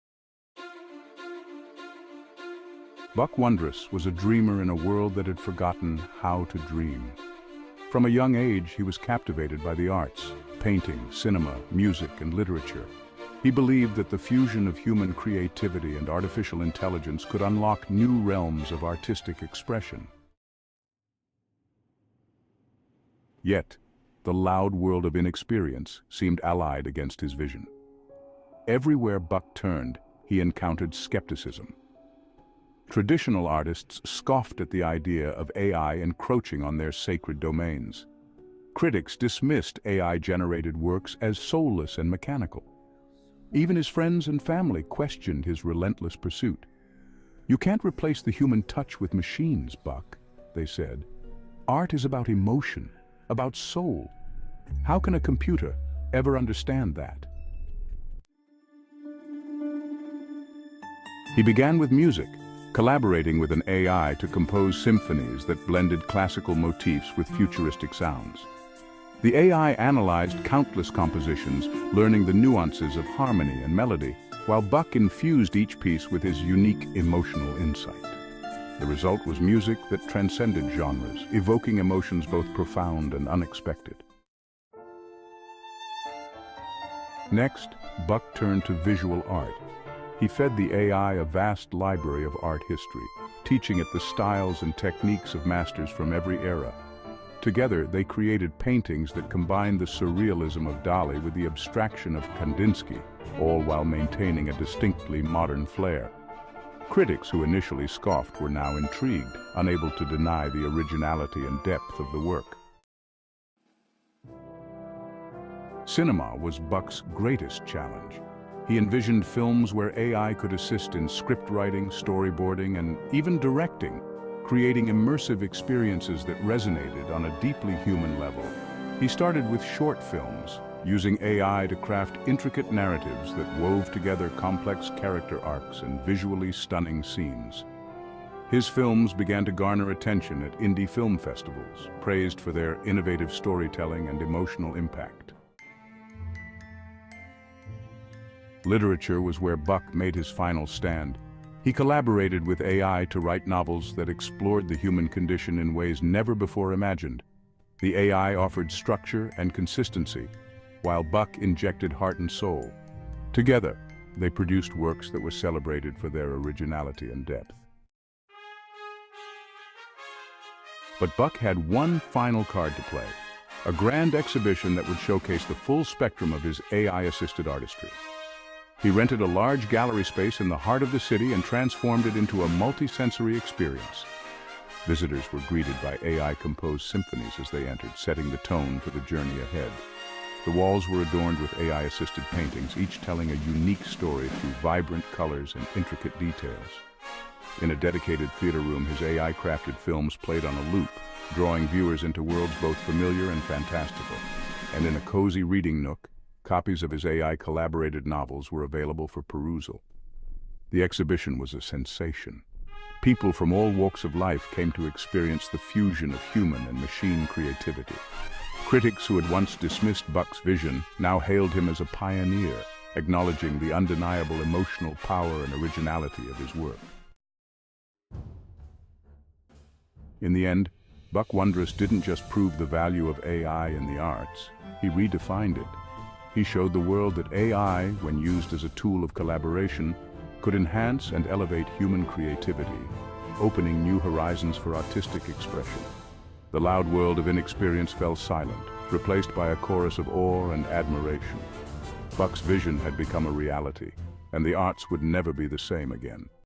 Buck Wondrous: AI Art Pioneer
For Buck Wondrous (Buckminster Wonderhouse) an Artist is, above all things, a creative visionary, not just a person with a brush in their hand. In fact, an Artist may have a quill or a baton in their hand...or a computer mouse. It is in the mind and spirit, not the appendages. A quadriplegic can be an Artist.